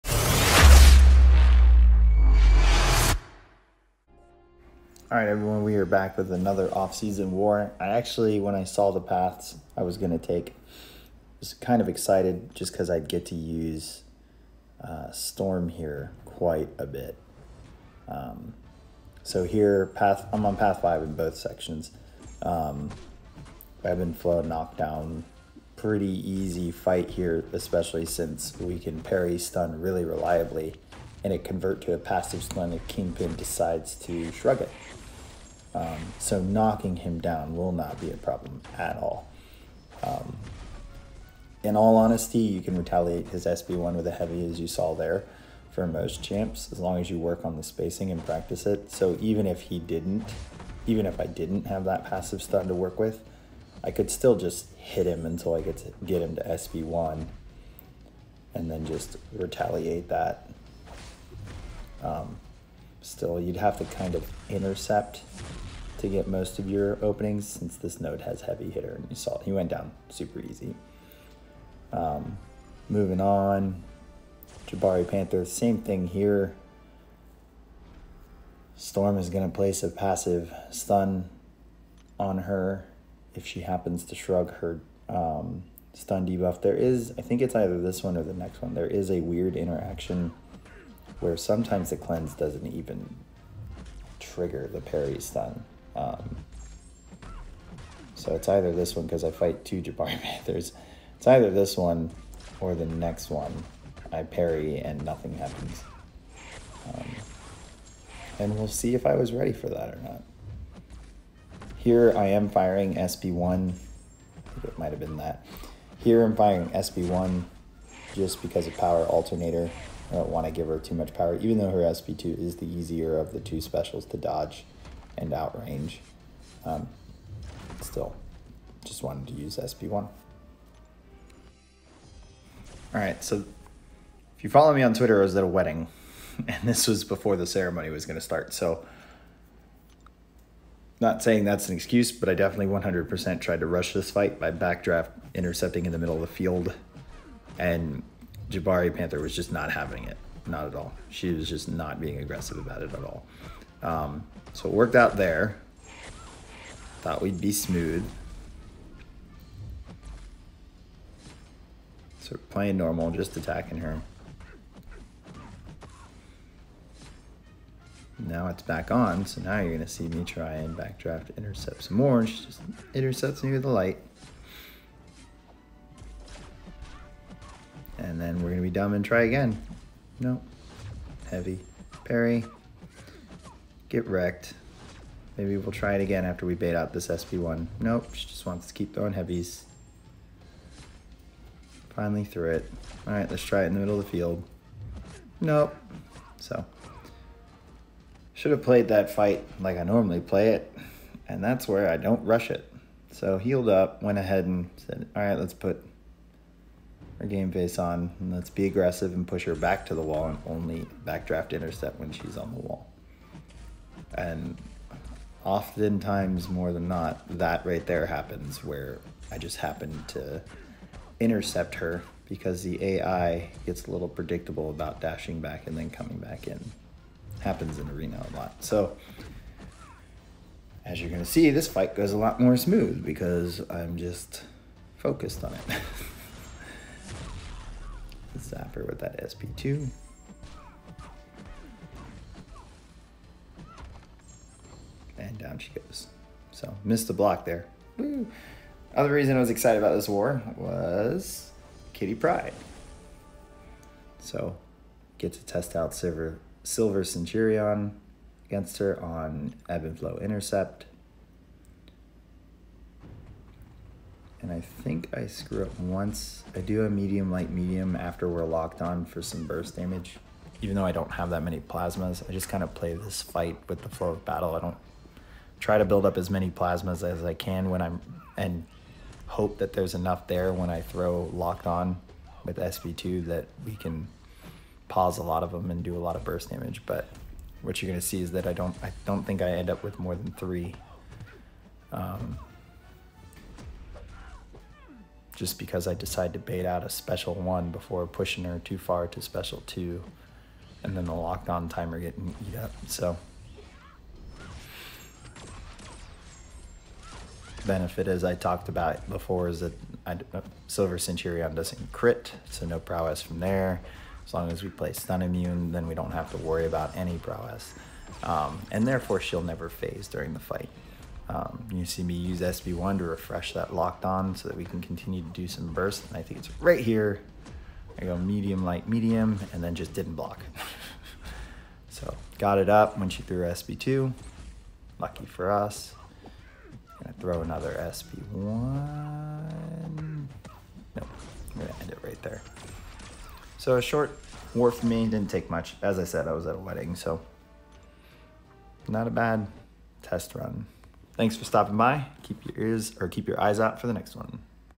MP Blaze (Off Season War 5)
MP Blaze takes 4 fights (4 path/shared)

Node 11 vs Kingpin
Node 19 vs Jabari Panther
Node 29 vs Jabari Panther
Node 37 vs Kitty Pryde